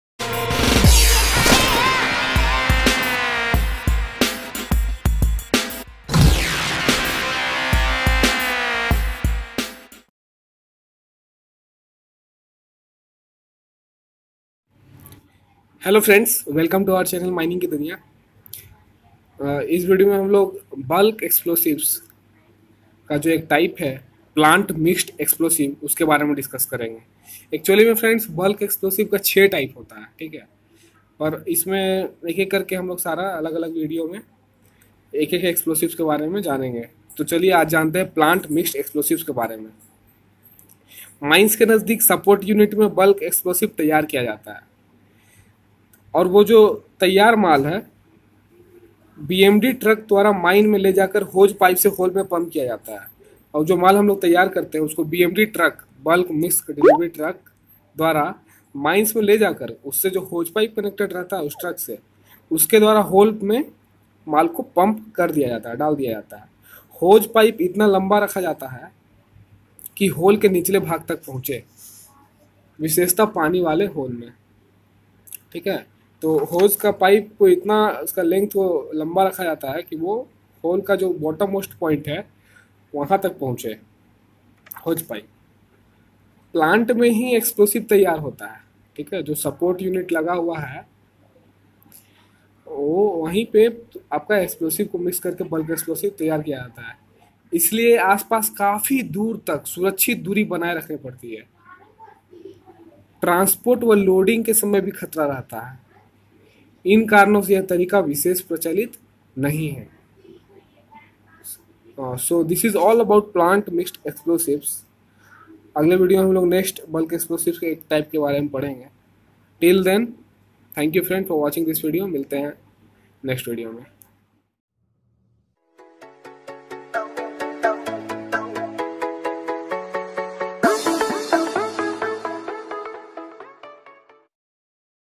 Plant 🌿 Mixed Explosive//Opencast mining /Blasting
This video is about plant Mixed Explosive a type of bulk explosive used in opencast mines.